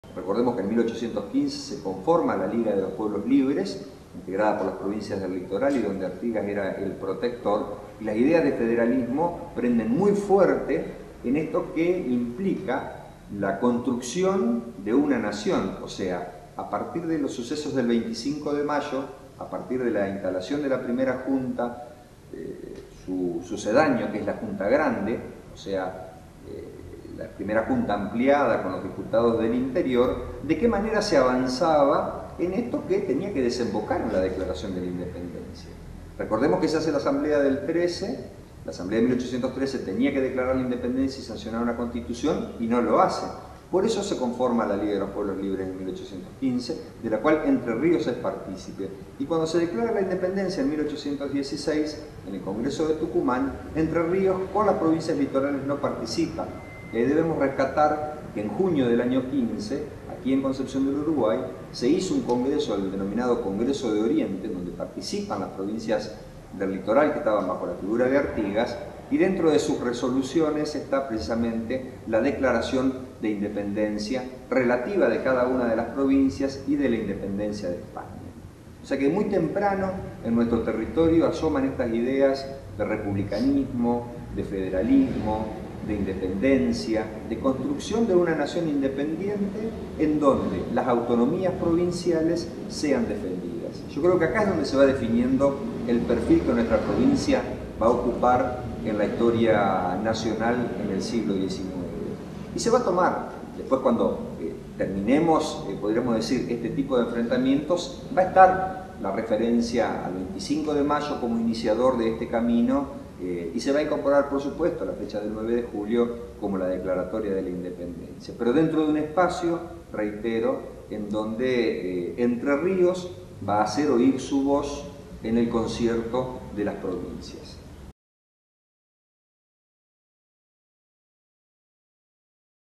9 de Julio de 1816 - Independencia - Por qué Entre Ríos no participa en el Congreso de Tucumán
Entrevista al Profesor Luis Angel Cerrudo, Director del Palacio San José, realizada por el equipo del Portal @prender, para el especial del 25 de Mayo.